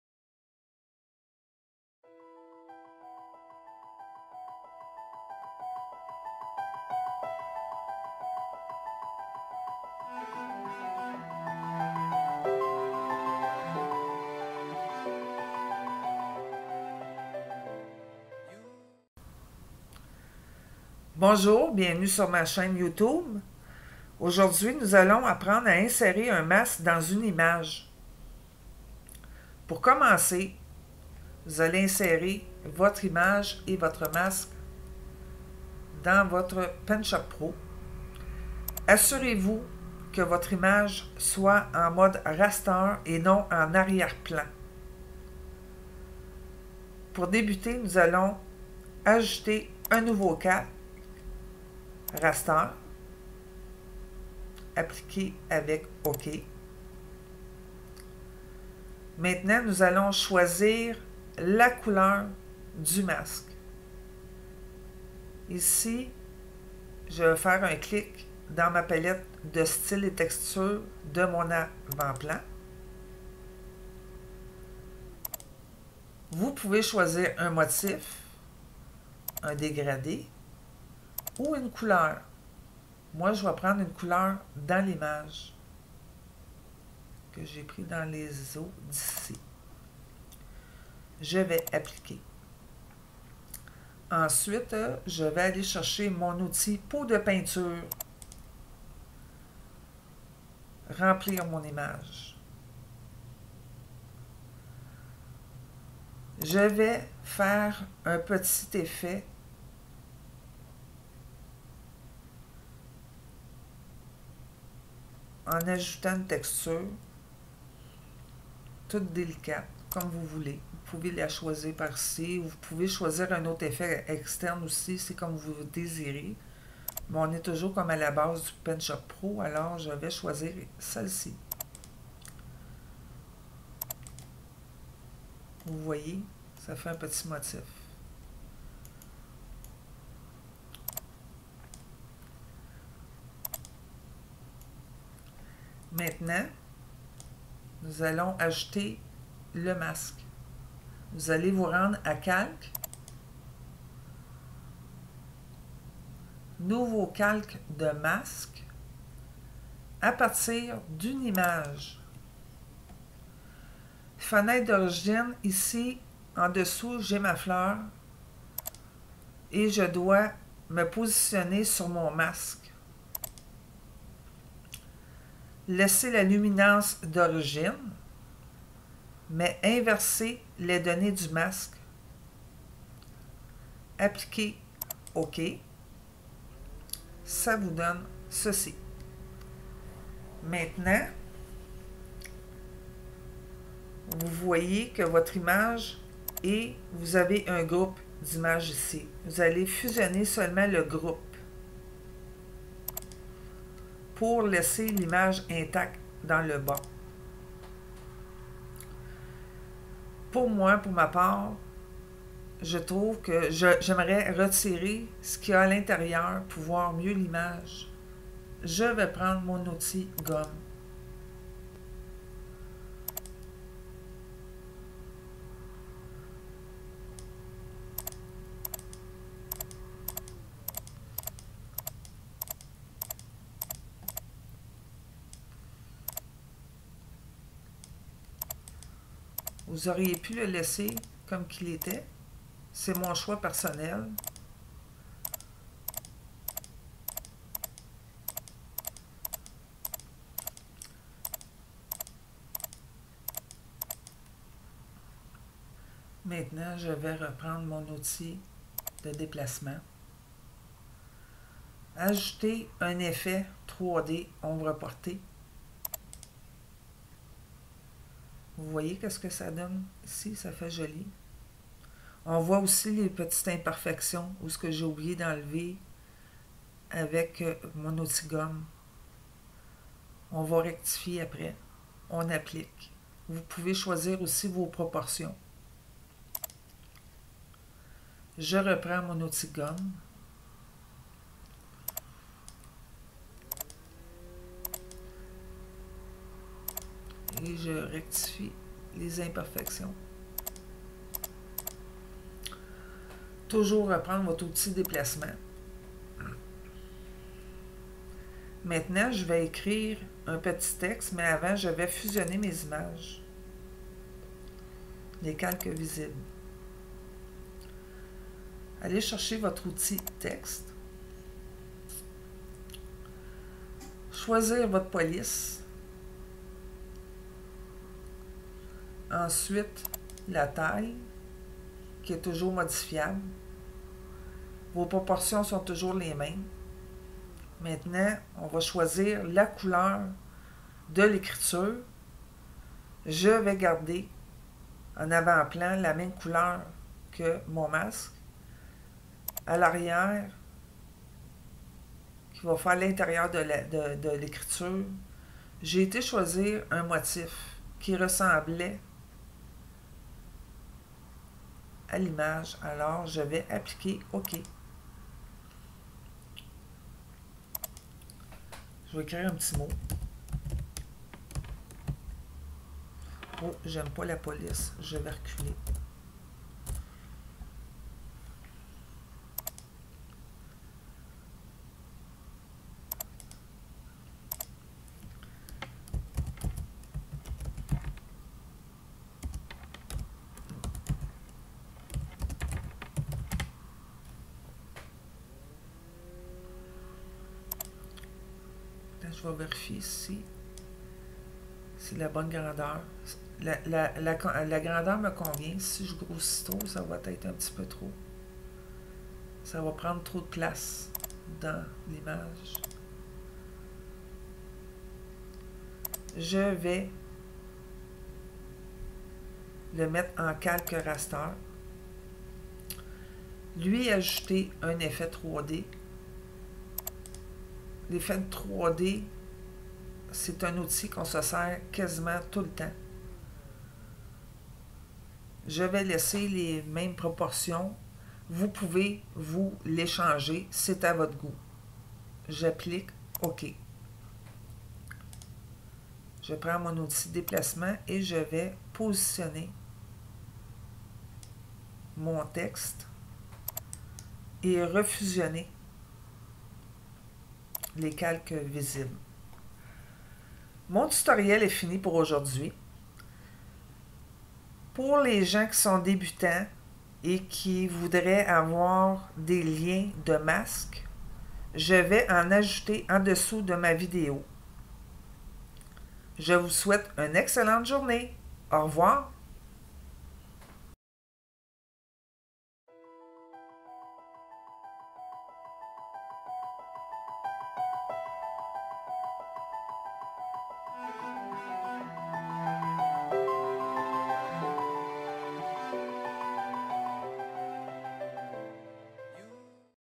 [ Tutoriel PaintShop Pro VF n°11 ] Comment ajouter un masque a une image avec Paint Shop Pro (PSP)
J'explique comment ajouter un masque dans une image
dans Paint Shop Pro ( PSP ).
Comme stipulé dans ma vidéo tutoriel,
Je vous est ajouté des liens si bas de masque (MASK).
PS: Vous êtes la bienvenue a en ajouter en commentaire.

Merci Suze.

Bonne journée Suze.